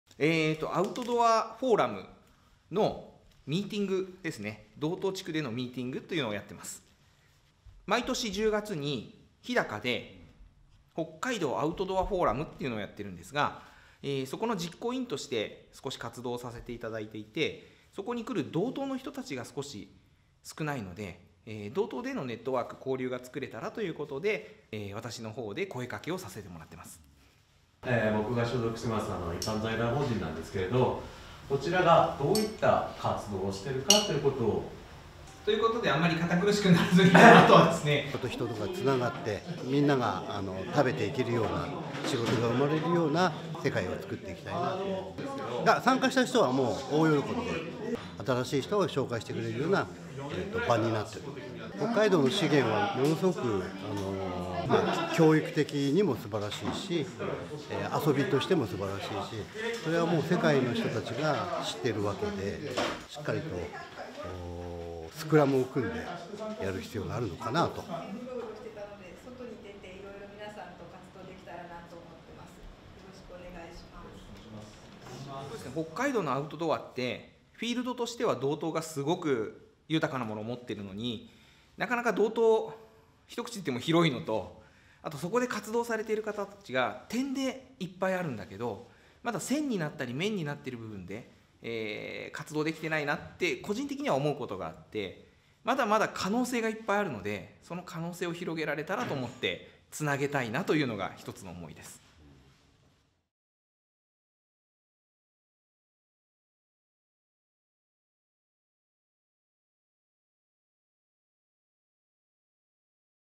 【4Ｋニュース】北海道アウトドアフォーラム・津別ミーティング
今年5回目を迎える「北海道アウトドアフォーラム」。北海道の魅力の一つであるアウトドアに関わる人たちが集まるイベントだ。
2019年3月5日6日、北海道津別町ランプの宿森つべつの研修室で、道東エリアのアウトドア事業者が集まり情報交換が行われた。
その模様をお送りする。